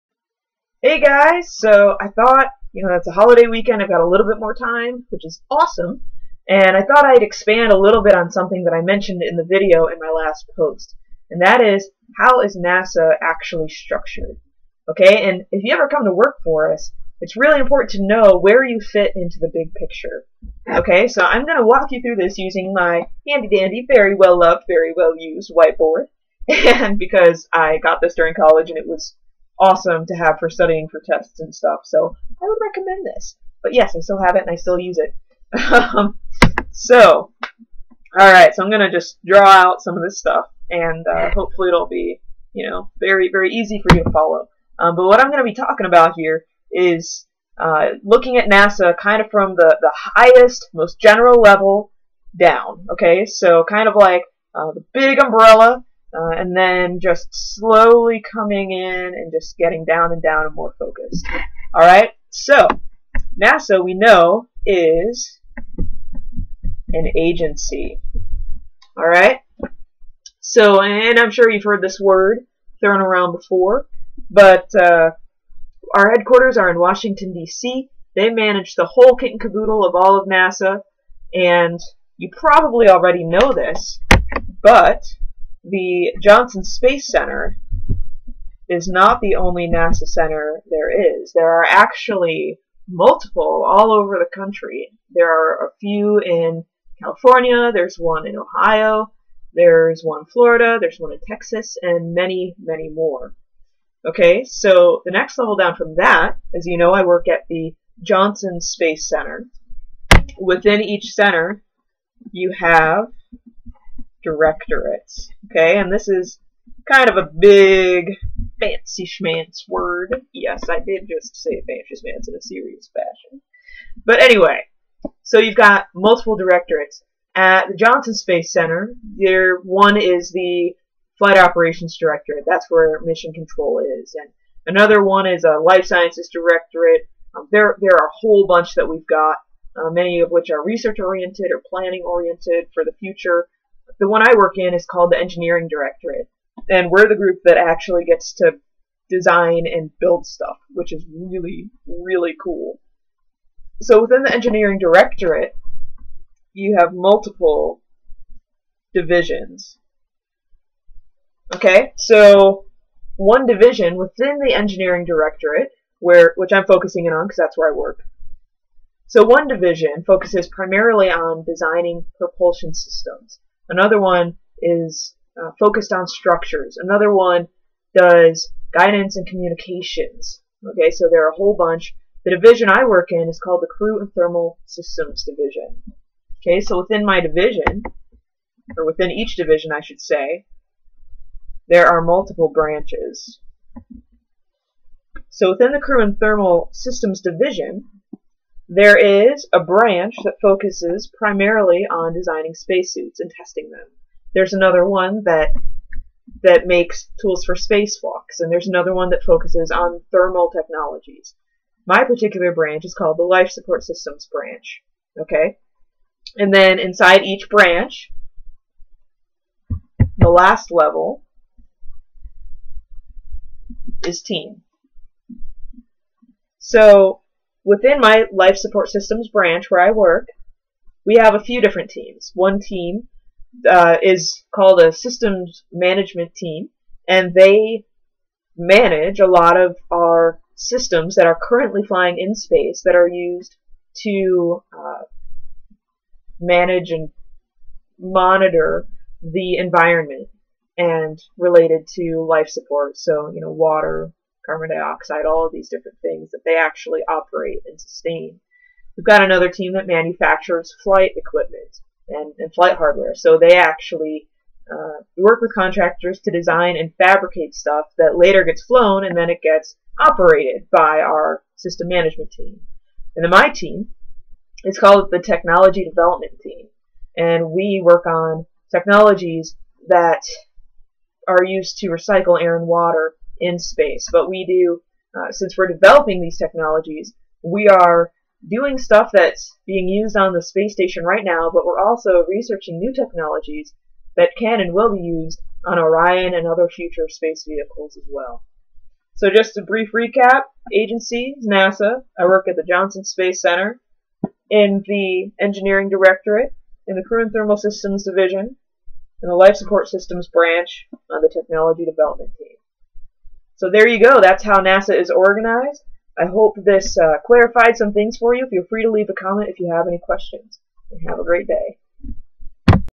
How is NASA Organized?
Description of the organization/structure/hierarchy of NASA, and where I fit into the big picture.